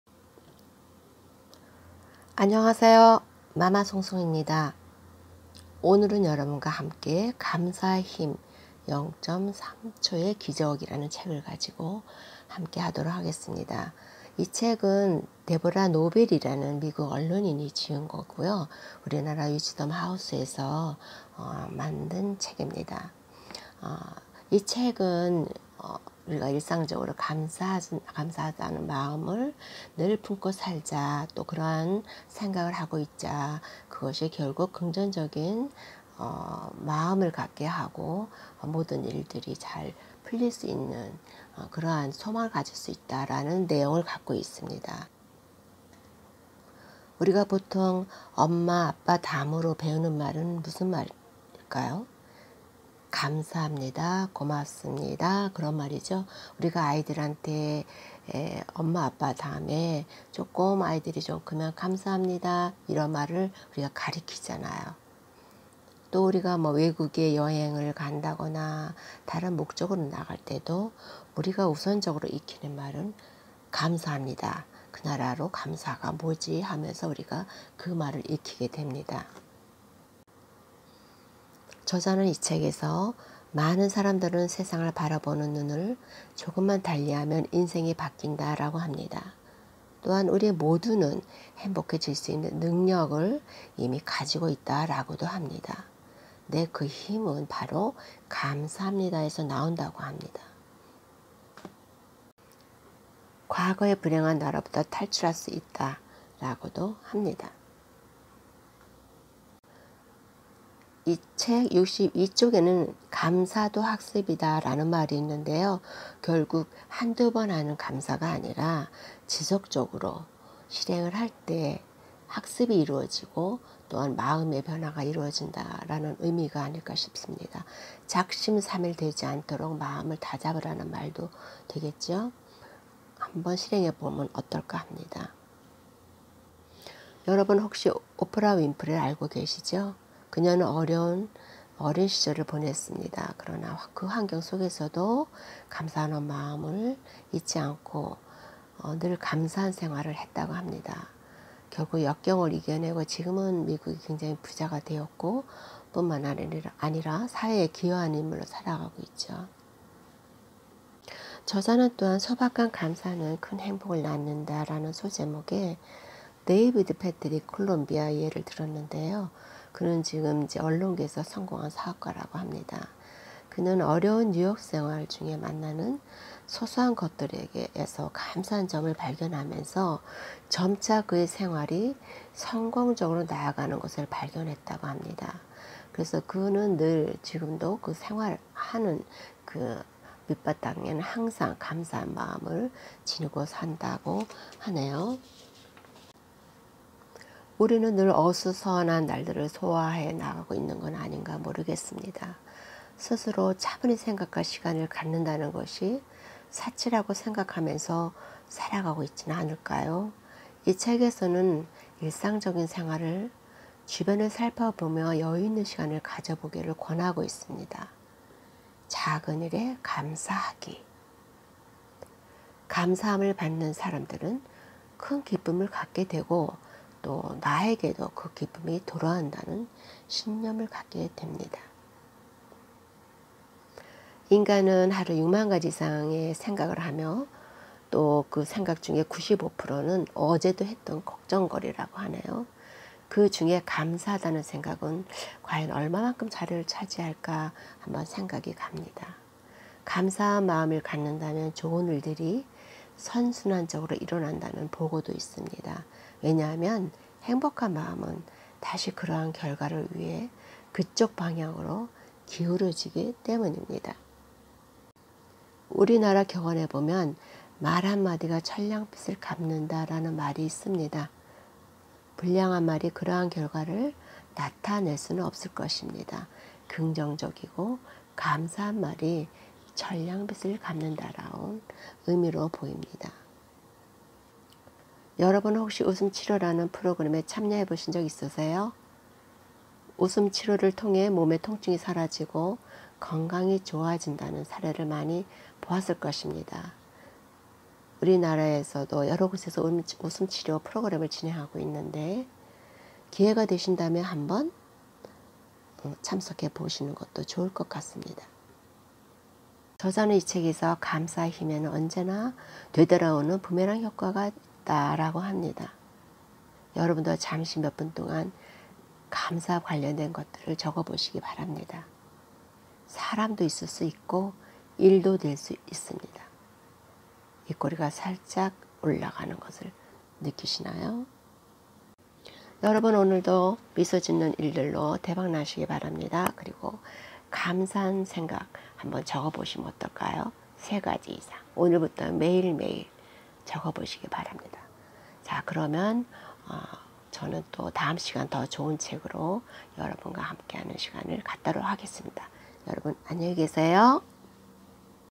[책과 함께] 세상을 보는 시선이 바뀌면 생활에 변화가 옵니다!! ｜감사의 힘
우리는 생활하면서 감사하기보다는 불평을 더 자연스럽게 합니다. 속이 확 풀리는 느낌을 만끽하기도 하지요. 자기 합리화도 맘을 위로합니다.
그런데 마음 한구석이 개운치 않은 경험을 하셨을 거예요. 왠지 모를 한 덩어리가 남아서 계속 맘을 불편하게 합니다.
그런데, 감사하는 생활은 얼굴에 웃음이 돌게 합니다.
오늘 함께 이 책을 공유하며 새로운 삶의 방향으로 출발해 볼까요?